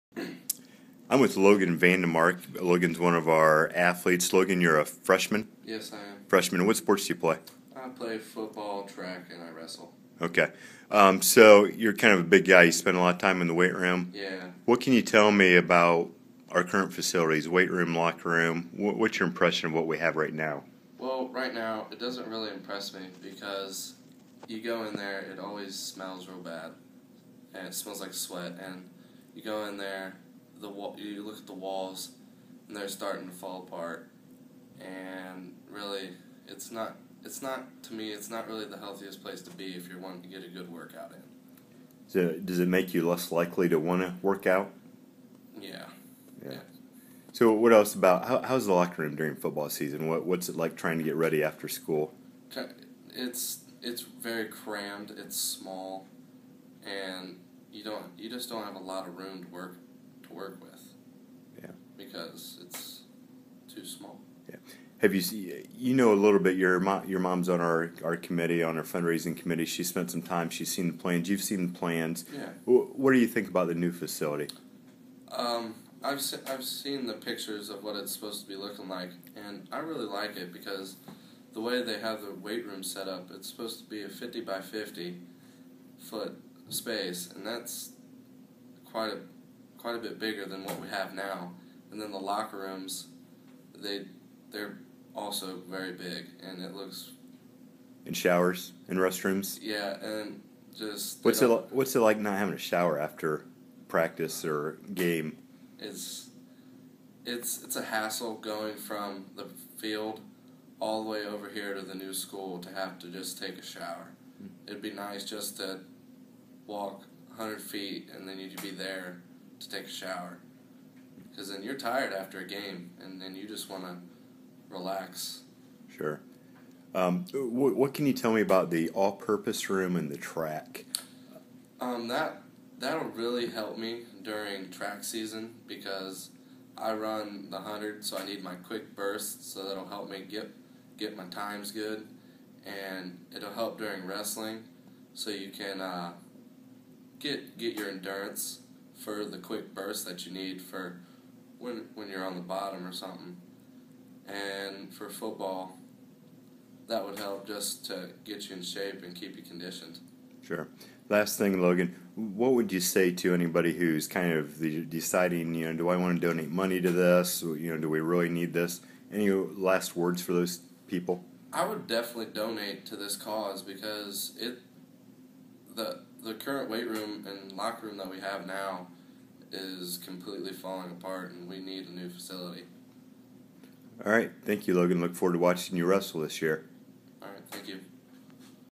Bearcat Strength and Training Center #5
Interview with freshman athlete Logan Vandemark.